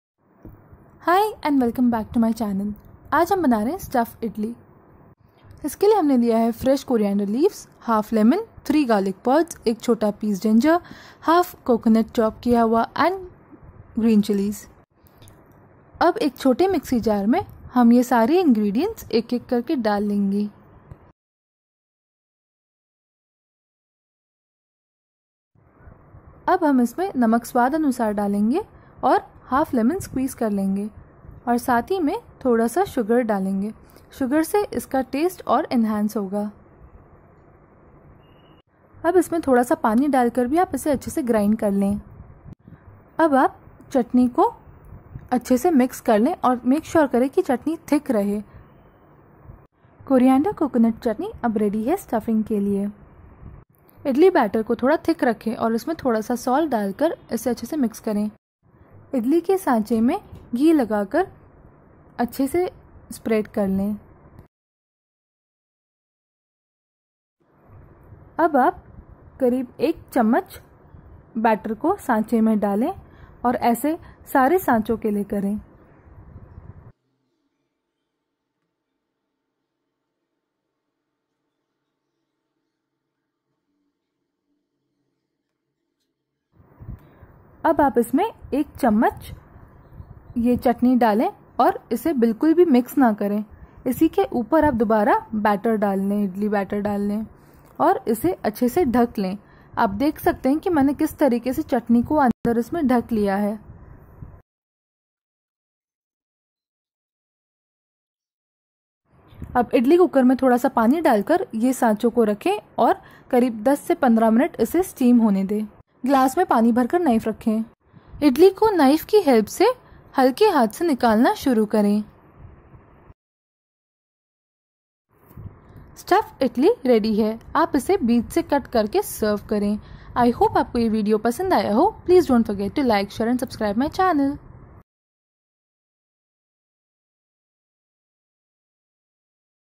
stuffed idli
Snacks
quick and easy recipe
good for takeaways for breakfast
i hope you like it